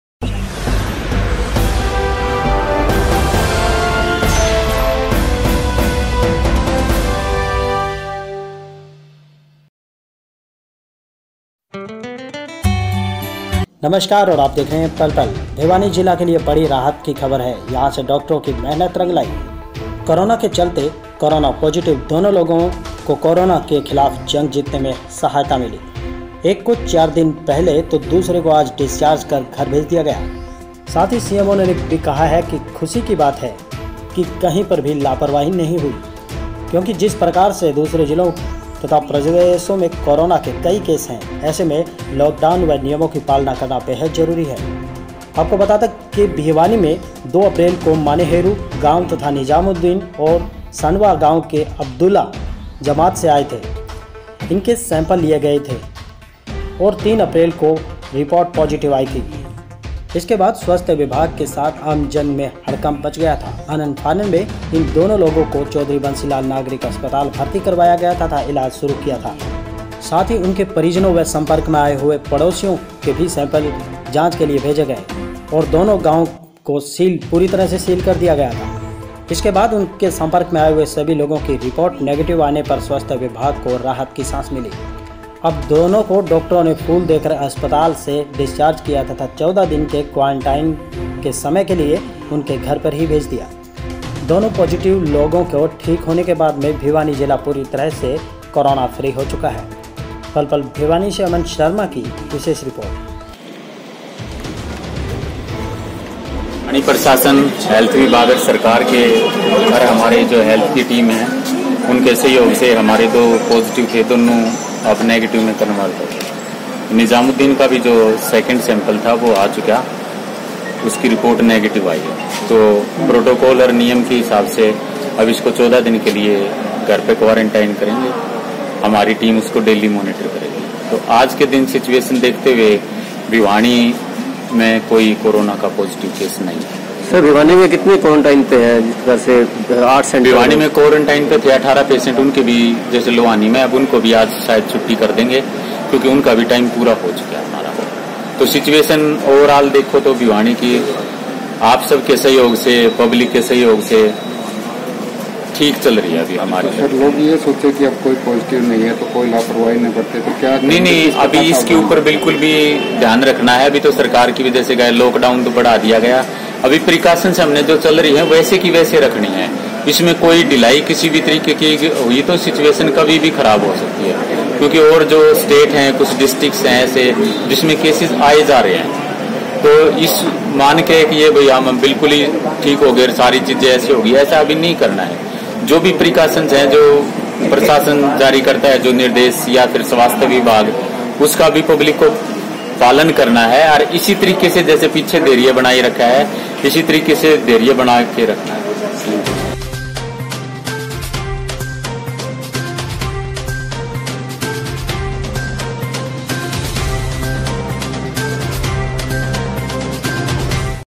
रंग लाई डॉक्टरों की मेहनत-सीएमओ ने कहा है कि ये खुशी की बात है लापरवाही नहीं हुई
भिवानी जिला के लिए बङी राहत की खबर है। यहां डोक्टरों की मेहनत
रंग लाई है जिसके चलते कोरोना पॉजिटिव दोनों लोगों ने कोरोना के खिलाफ जंग जीत
ली है। एक को चार रोज पहले और दूसरे को आज डिस्चार्ज कर घर भेज दिया है। साथ
ही सीएमओ ने कहा है कि ये खुशी की बात है लापरवाही की नहीं, क्योंकि दूसरे
जिलों व प्रदेशों में कोरोना के कई केस हैं। ऐसे में लॉकडाउन व नियमों की
पालना करना जरूरी है।